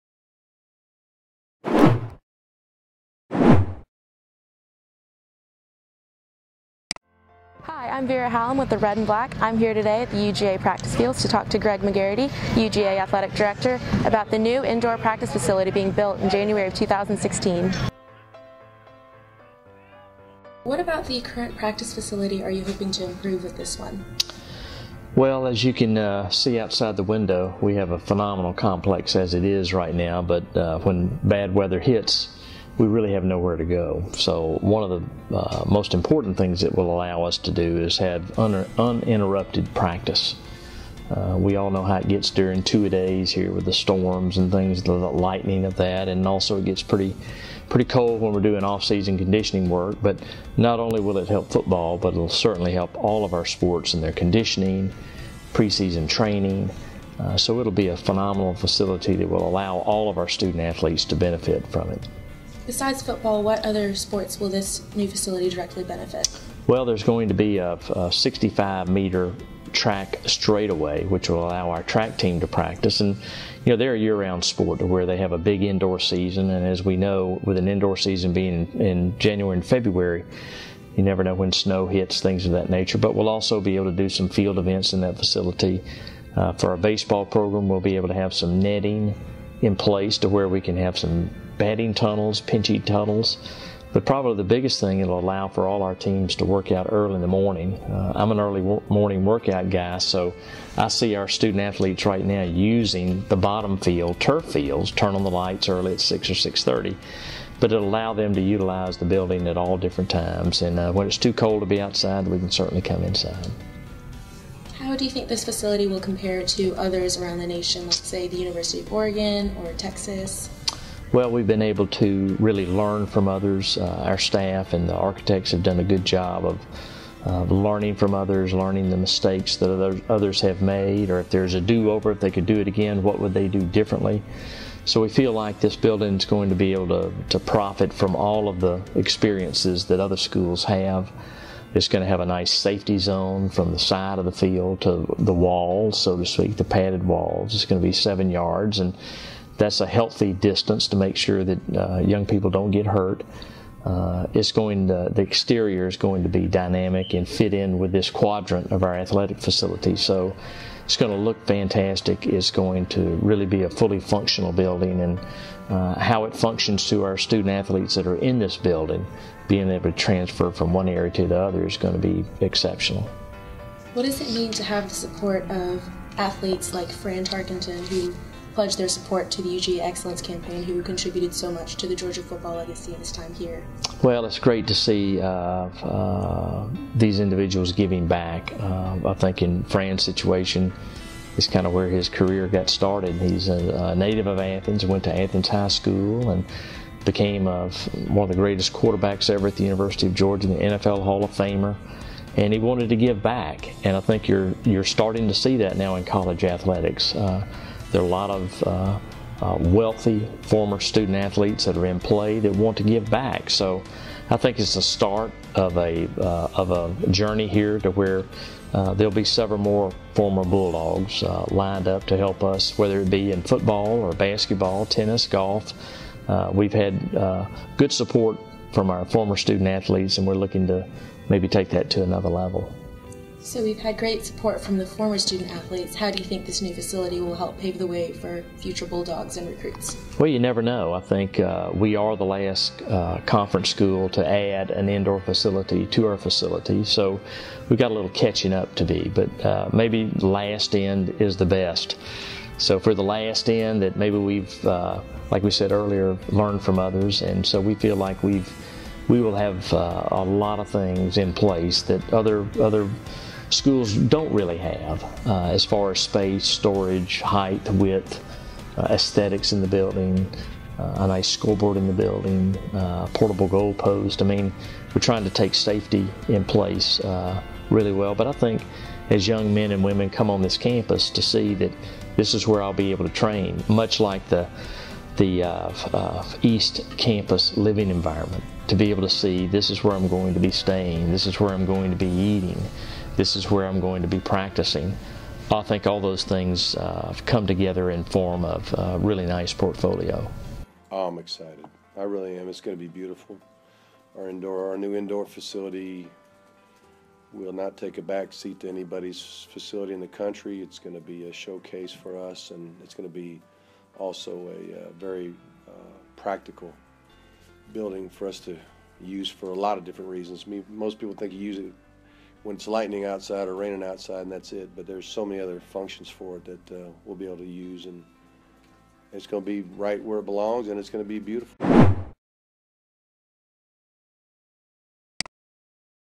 McGarity & Richt talk about UGA's indoor sports facility
Vira Hilam asks University of Georgia Athletic Director Greg Mcgarity and Head Coach Mark Richt about the indoor practice facility to be built in January 2016.
BY VIRA HILAM